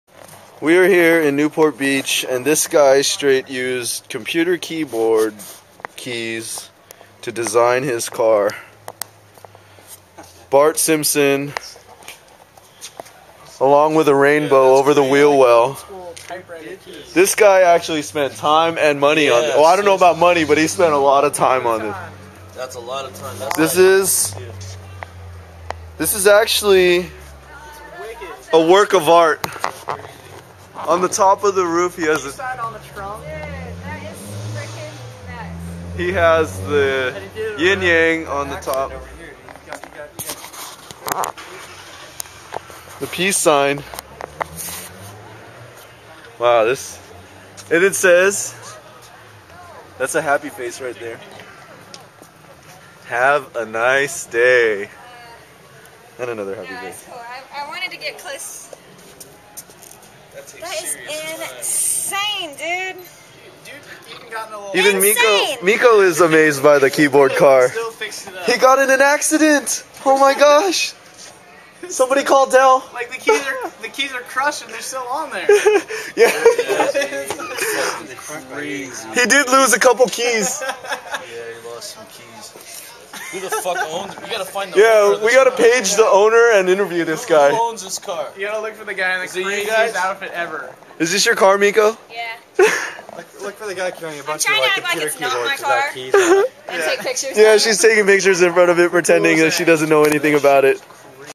Cool keyboard car w Meiko @ Newport Beach Film Festival Afterparty 042409
you have to check out this crazy car covered with keyboard or typewriter keys. It's pretty dope! At the Newport Beach Film Festival Afterparty 042409